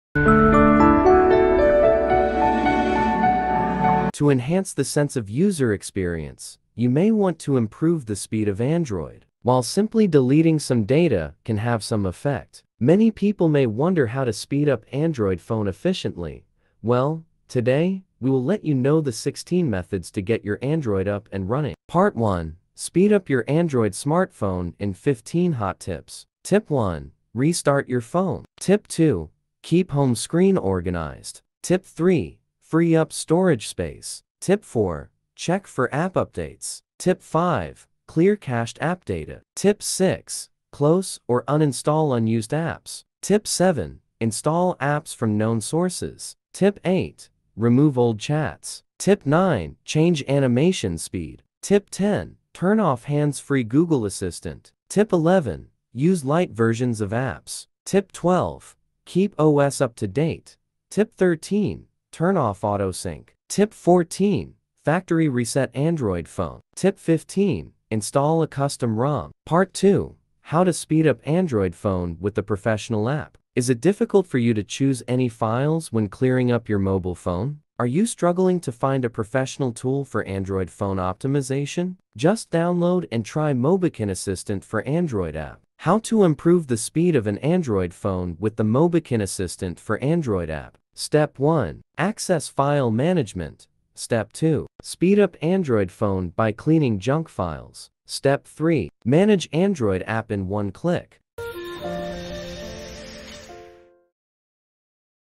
16 Tips & Tricks to Speed Up Android Phone [Definitive Guide]
How can I speed up my Android phone with the most common approachs? Focus on this definitive guide to learn how to speed up Android phone in 16 optional ways. And you will surely be awarded.

00:00 Start
00:21 Part 1: Speed Up Your Android Smartphone in 15 Hot Tips
01:09 Part 2: How to Speed Up Android Phone with a Professional App